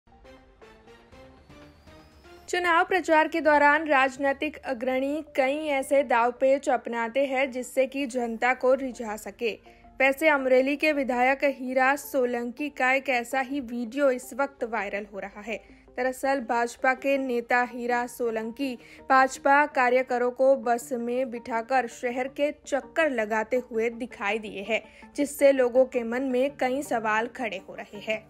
अमरेली के विधायक हीरा सोलंकी ने भाजपा कार्यकरो को बस में बिठाकर घुमाया
BRG NEWS  App: